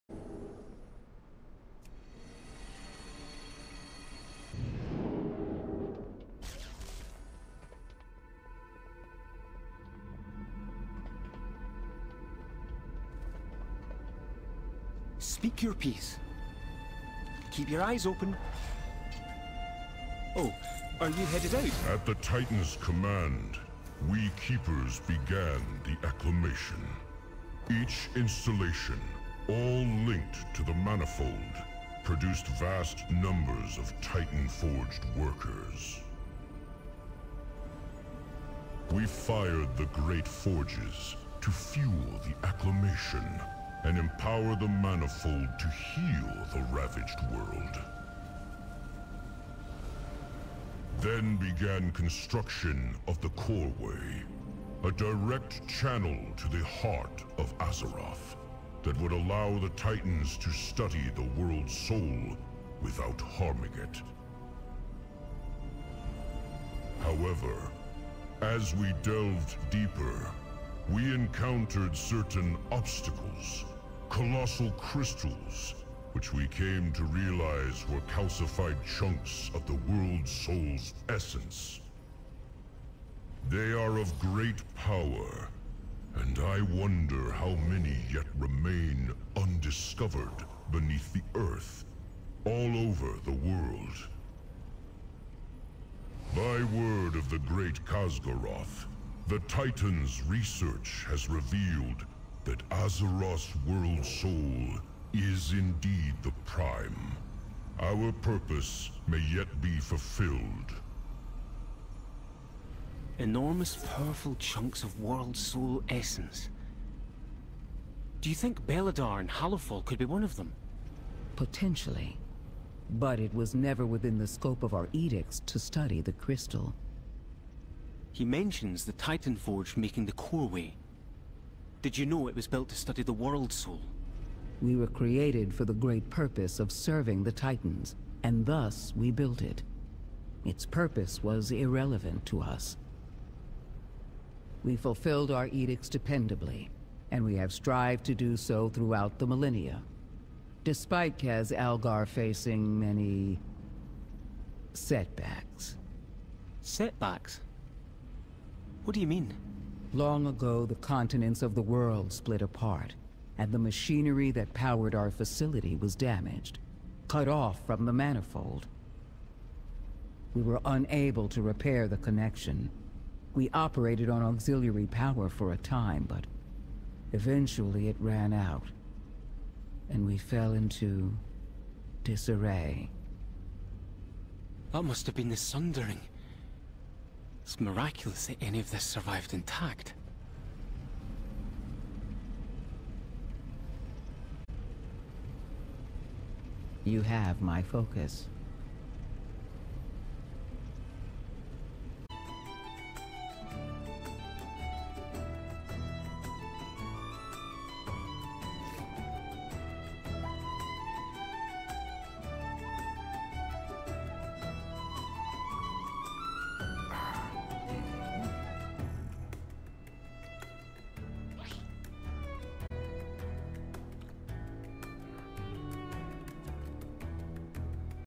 Highlight: More disks for Dagran pt 3 - Manifolds and chunks of worldsoul
We give another 100 disks to Dagran and Brinthe and find out a little bit more about the Manifold, the Coreway and chunks of worldsoul of which they hint there could be many... Beledar among them. They do not state outright that Beledar is that, but it's pretty obvious.